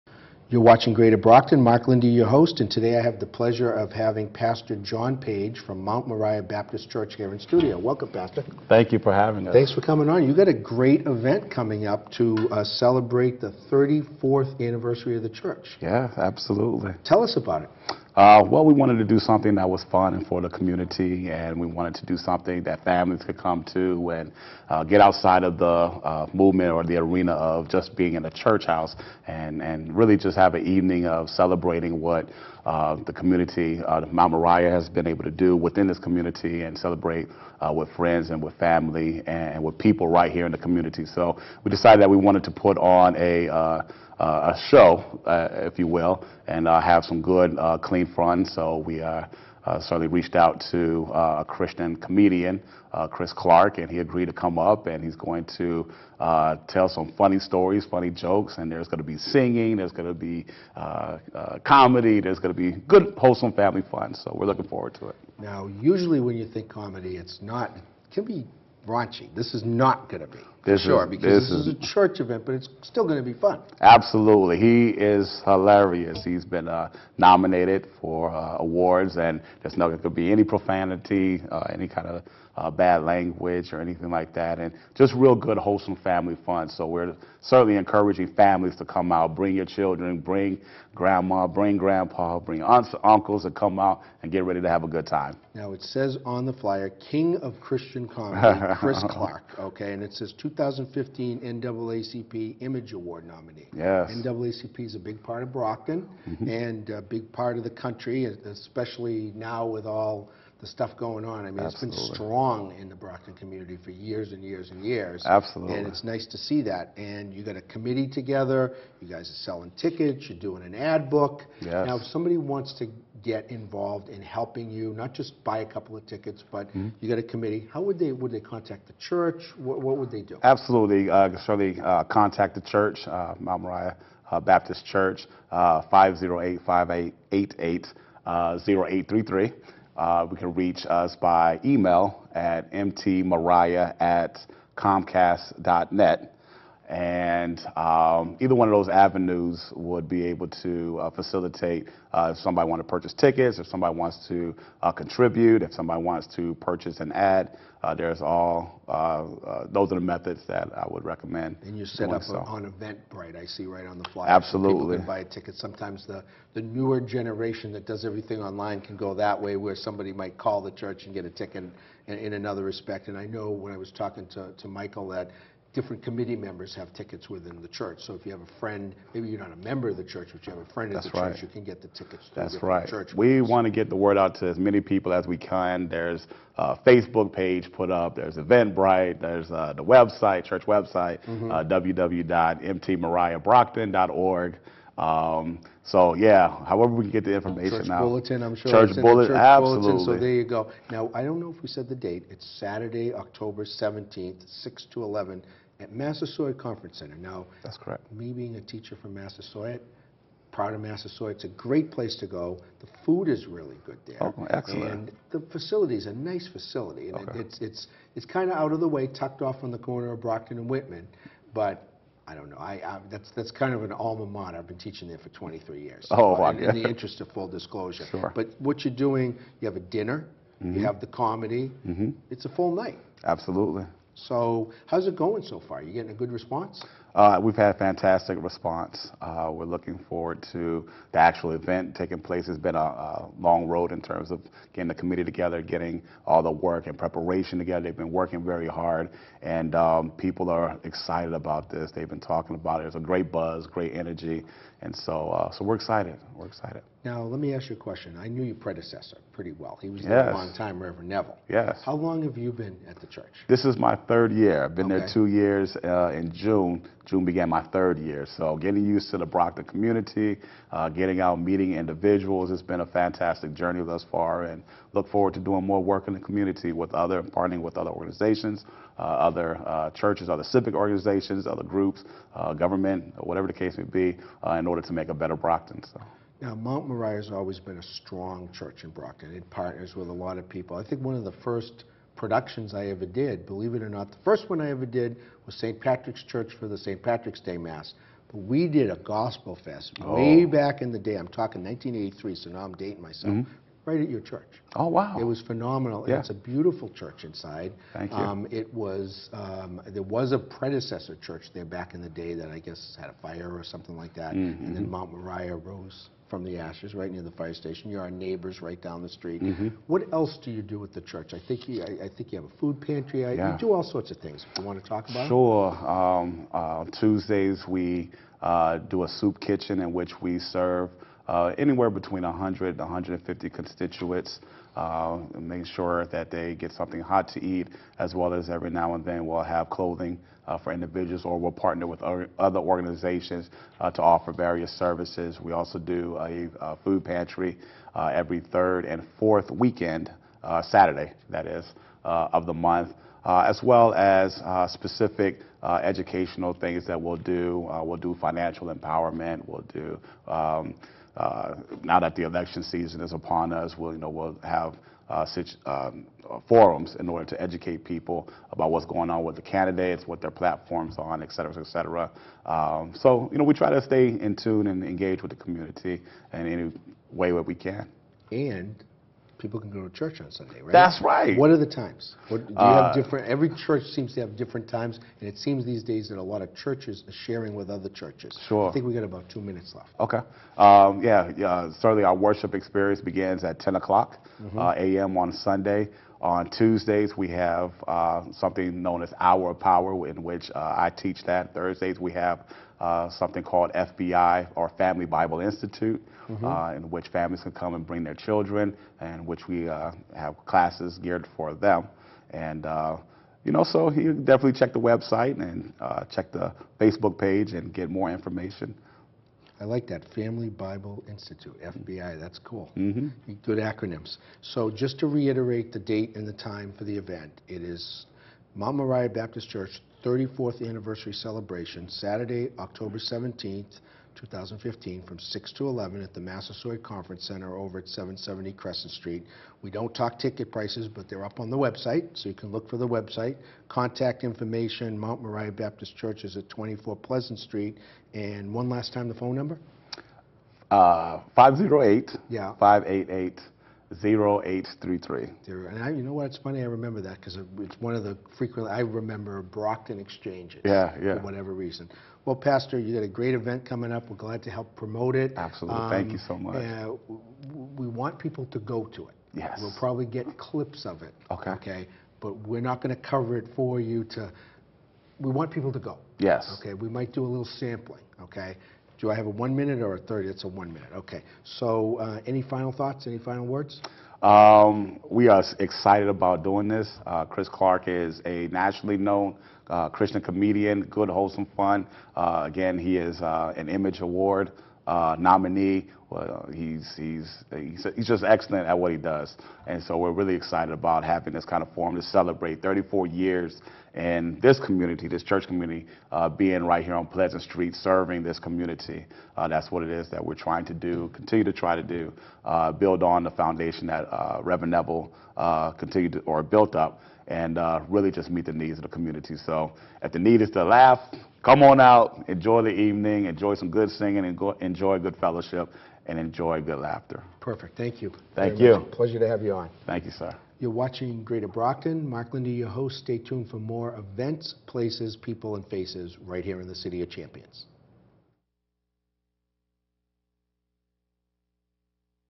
Greater Brockton - John Page
Pastor John Paige from Mt. Moriah Baptist talks about a Fundraising event on October 17 at the Massasoit Conference center